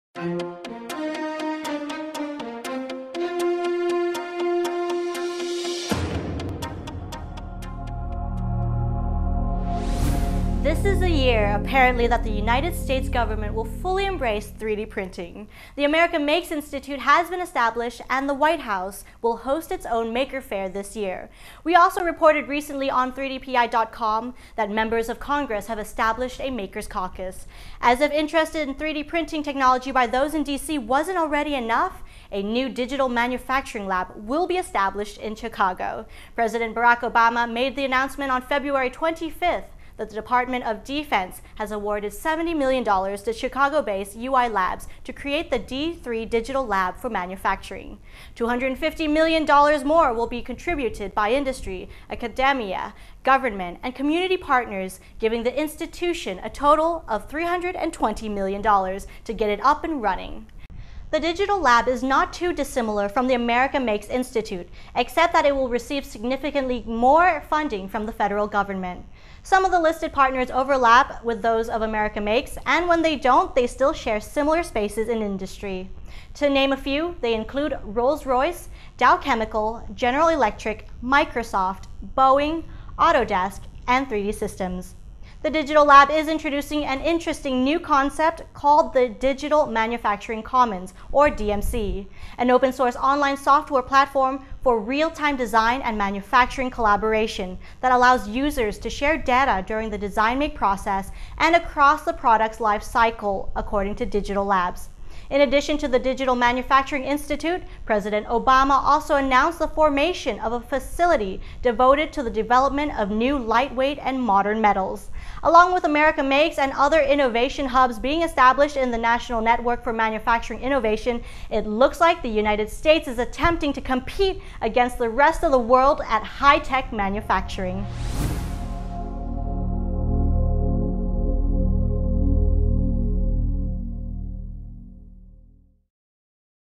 Obama Announces Two New Advanced Manufacturing Hubs
Video Transcript: This is the year, apparently, that the United States government will fully embrace 3D printing. The America Makes Institute has been established and the White House will host its own Maker Faire this year. We also reported recently on 3DPI.com that members of Congress have established a Makers Caucus. As if interest in 3D printing technology by those in DC wasn't already enough, a new digital manufacturing lab will be established in Chicago.

President Barack Obama made the announcement on February 25 that the Department of Defense has awarded $70 million to Chicago-based UI Labs to create the D3digital Lab for Manufacturing. $250 million more will be contributed by industry, academia, government and community partners, giving the institution a total of $320 million to get it up and running.

The Digital Lab is not too dissimilar from the America Makes institute, except that it will receive significantly more funding from the federal government. Some of the listed partners overlap with those of America Makes and, when they don't, they still share similar spaces in industry. To name a few, they include: Rolls Royce, Dow Chemical, General Electric, Microsoft, Boeing, Autodesk and 3D Systems. The Digital Lab is introducing an interesting new concept called the Digital Manufacturing Commons (DMC), an open source online software platform  "for real-time design and manufacturing collaboration" that "allows users to share data during the design-make process and across the product life cycle," according to Digital Lab's.

In addition to the digital manufacturing institute, President Obama also announced the formation of a facility devoted to the development of new lightweight and modern metals.

Along with America Makes and the other Innovation Hubs being established in the National Network for Manufacturing Innovation, it looks like the United States is attempting to compete against the rest of the world at high tech manufacturing.